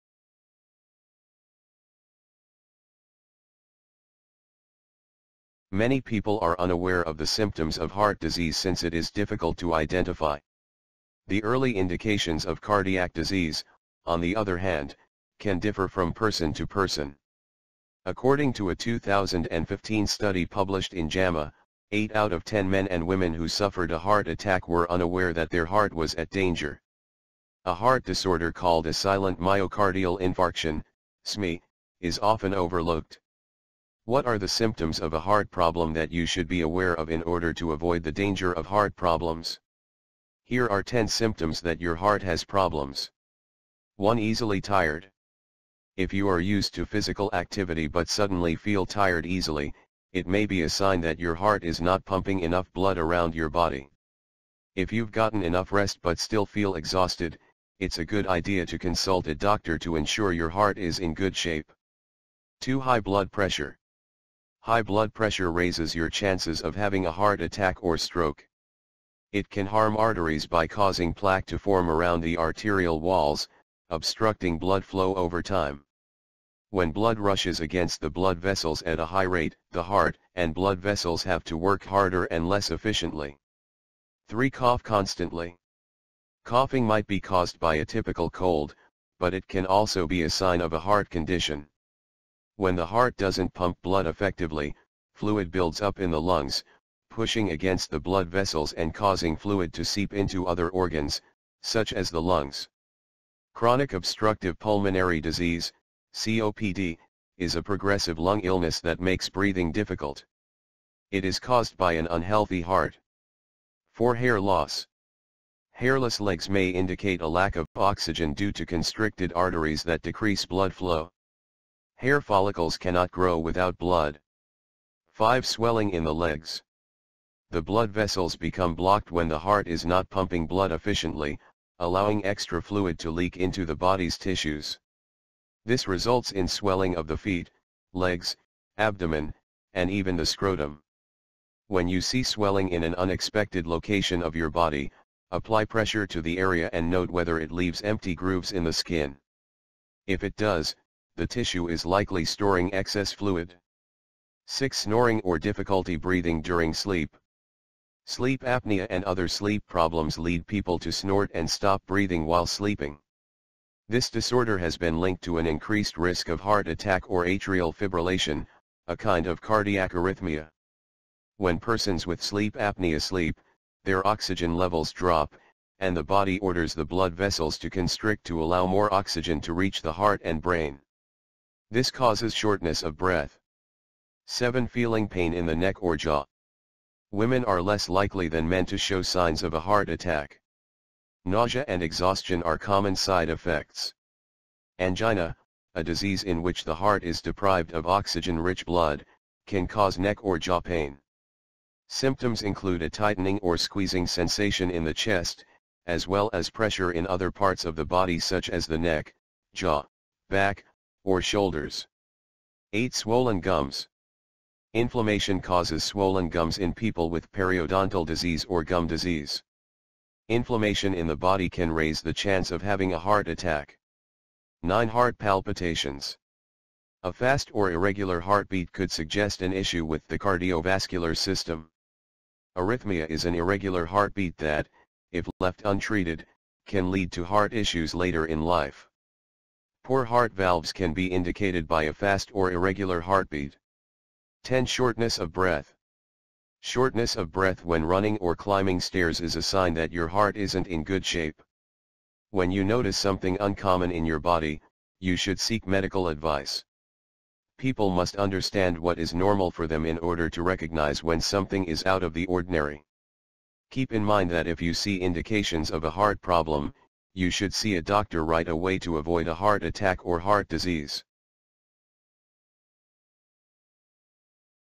Important Signs Your Heart is in Trouble
If you are used to physical activity but suddenly feel tired easily, it may be a sign that your heart is not pumping enough blood around your body.

If you've gotten enough rest but still feel exhausted, it's a good idea to consult a doctor to ensure your heart is in good shape.

High blood pressure raises your chances of having a heart attack or stroke. It can harm arteries by causing plaque to form around the arterial walls, obstructing blood flow over time.

When blood rushes against the blood vessels at a high rate, the heart and blood vessels have to work harder and less efficiently.

Coughing might be caused by a typical cold, but it can also be a sign of a heart condition.

When the heart doesn't pump blood effectively, fluid builds up in the lungs, pushing against the blood vessels and causing fluid to seep into other organs, such as the lungs. Chronic obstructive pulmonary disease (COPD) is a progressive lung illness that makes breathing difficult. It is caused by an unhealthy heart.

Hairless legs may indicate a lack of oxygen due to constricted arteries that decrease blood flow. Hair follicles cannot grow without blood.

The blood vessels become blocked when the heart is not pumping blood efficiently, allowing extra fluid to leak into the body's tissues. This results in swelling of the feet, legs, abdomen, and even the scrotum.

When you see swelling in an unexpected location of your body, apply pressure to the area and note whether it leaves empty grooves in the skin. If it does, the tissue is likely storing excess fluid.

Sleep apnea and other sleep problems lead people to snort and stop breathing while sleeping. This disorder has been linked to an increased risk of heart attack or atrial fibrillation, a kind of cardiac arrhythmia.

 When persons with sleep apnea sleep, their oxygen levels drop, and the body orders the blood vessels to constrict to allow more oxygen to reach the heart and brain. This causes shortness of breath.

Women are less likely than men to show signs of a heart attack. Nausea and exhaustion are common side effects.

Angina, a disease in which the heart is deprived of oxygen-rich blood, can cause neck or jaw pain. Symptoms include a tightening or squeezing sensation in the chest, as well as pressure in other parts of the body such as the neck, jaw, back, or shoulders.

Inflammation causes swollen gums in people with periodontal disease or gum disease. Inflammation in the body can raise the chance of having a heart attack.

A fast or irregular heartbeat could suggest an issue with the cardiovascular system.

Arrhythmia is an irregular heartbeat that, if left untreated, can lead to heart issues later in life. Poor heart valves can be indicated by a fast or irregular heartbeat.

Shortness of breath when running or climbing stairs is a sign that your heart isn't in good shape. When you notice something uncommon in your body, you should seek medical advice. People must understand what is normal for them in order to recognize when something is out of the ordinary.